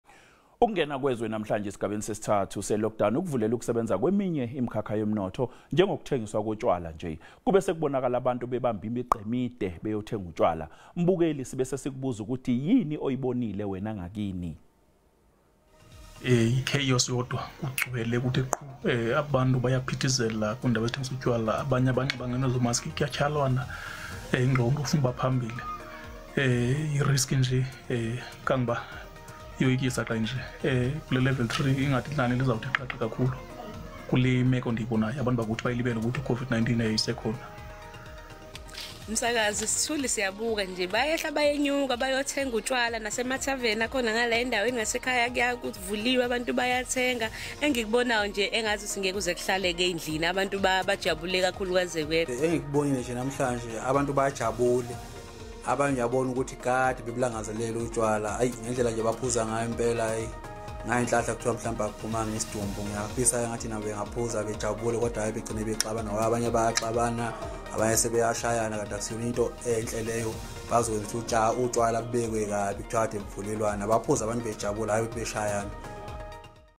Imibono yababukeli @7pm | 01 June 2020
Ukungena kwezwe namhlanje esigabeni sesithathu selockdown kuvulele ukusebenza kweminye imikhakha yomnotho njengokuthengiswa kotshwala nje. Kube sekubonakala abantu bebambe imigqa emide beyothenga utshwala. Mbukeli sibe sesikubuza ukuthi yini oyibonile wena ngakini?

For more news, visit sabcnews.com and also SABCNews SABCIzindaba Imibono on Social Media.